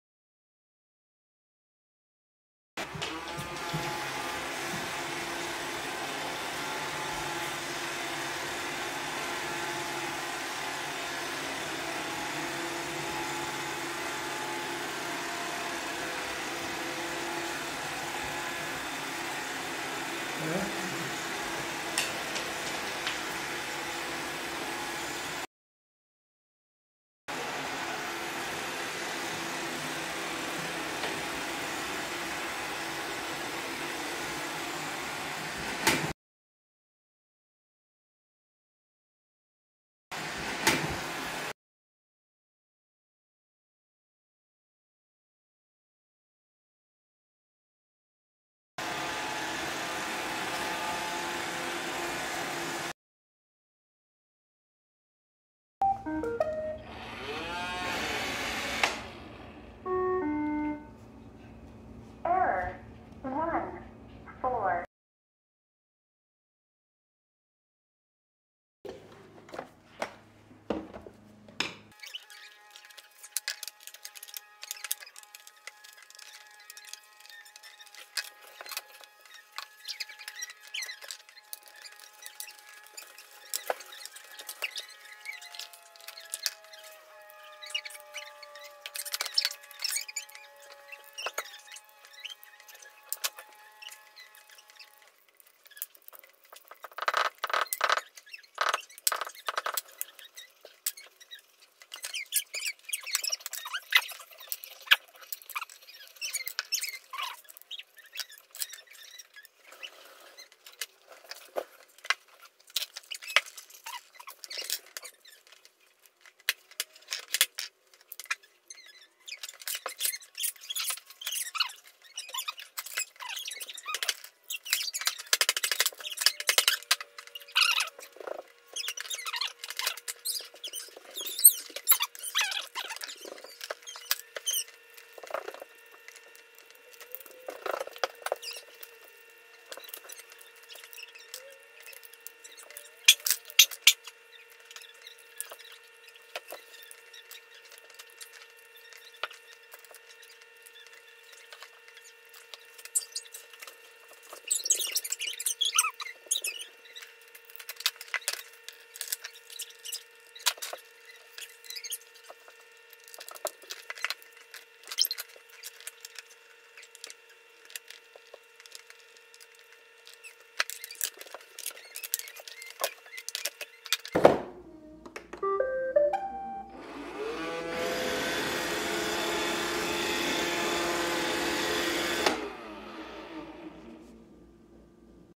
Caucasian Shepherd With Roomba Robot. Dog vs Robot, Shedding vs Tech :)
Caucasian Shepherd in Canada